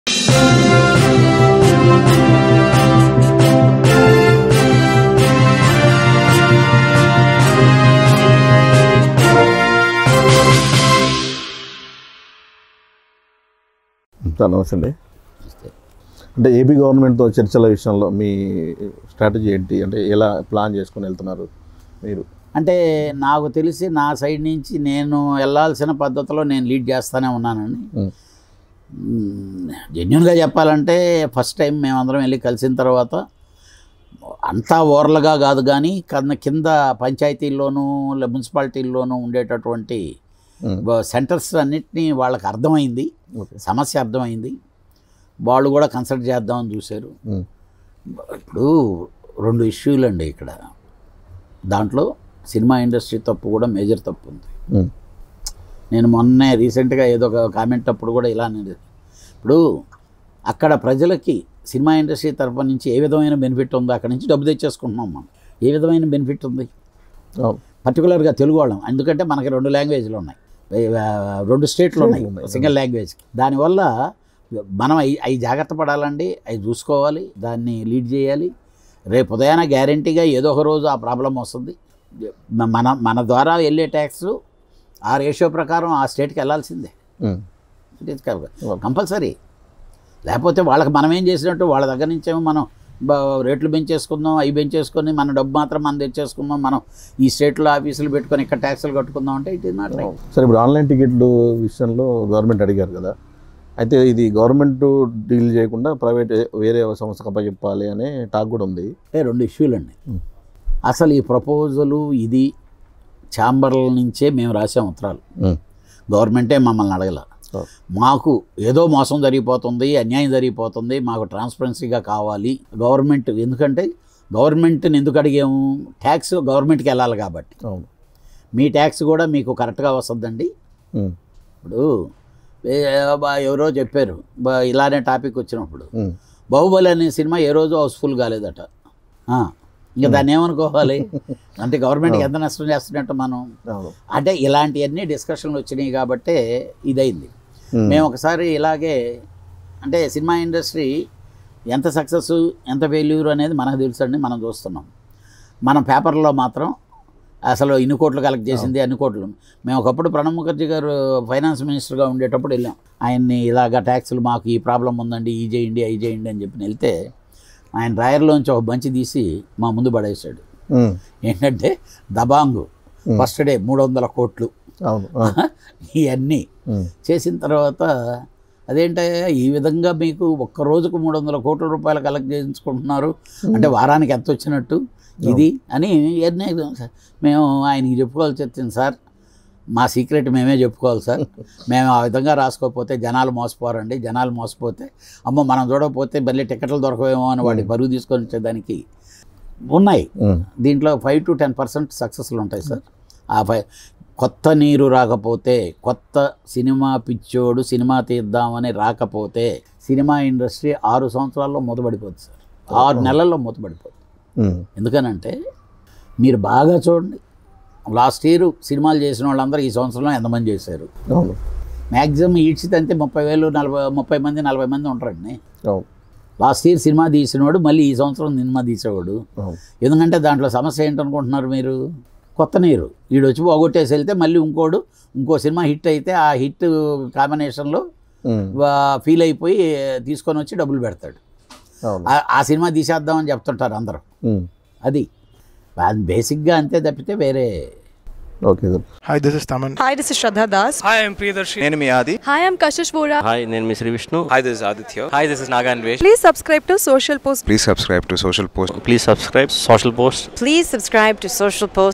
Producer C Kalyan Sensational Comments on Movie Tickets Price in AP || Producer C Kalyan Interview
Producer C Kalyan Sensational Comments on Movie Tickets Price in AP || Producer C Kalyan Interview || SocialPostTv

Hello Friends,